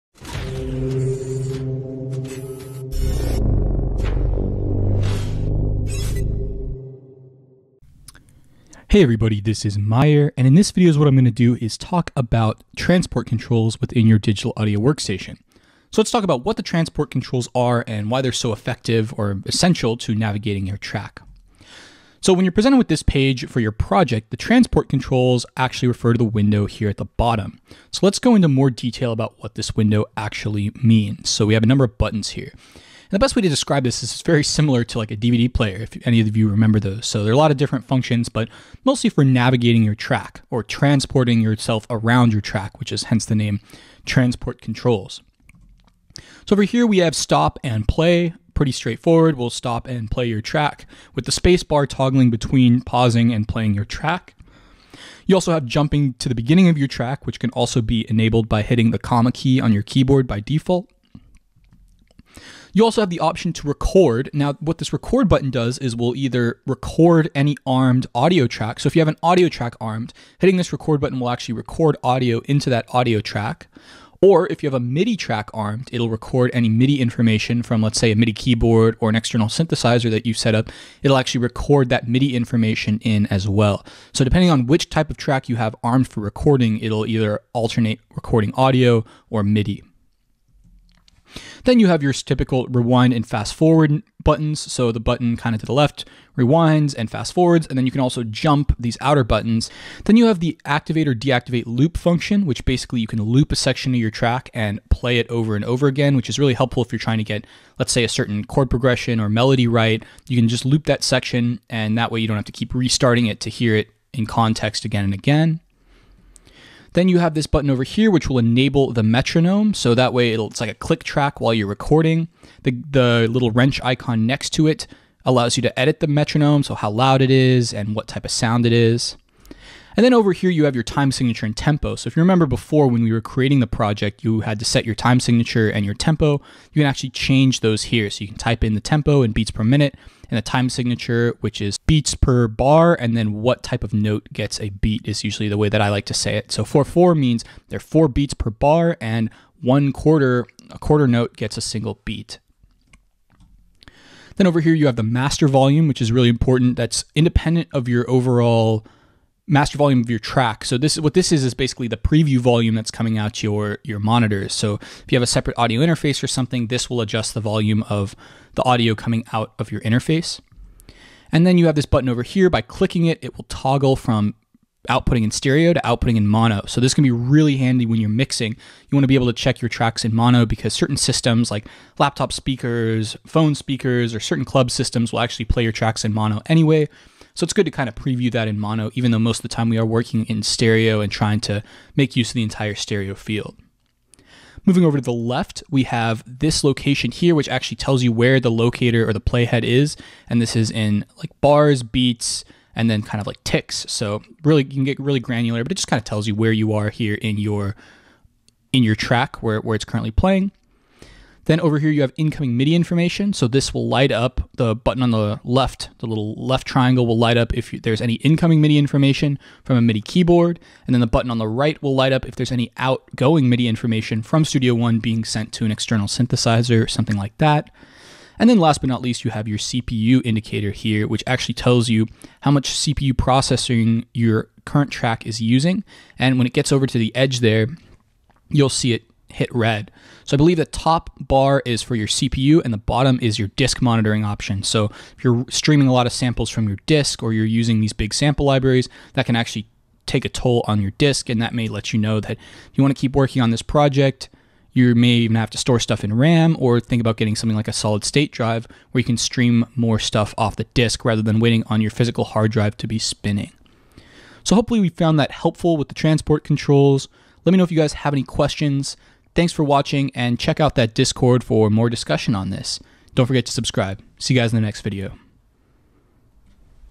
Intro to Music Production [Module 2.2] - Transport Controls
In this video, I'll talk more about the transport controls in your DAW and how to navigate your track with them.  This is very similar to the buttons on your DVD player or remote when watching a movie.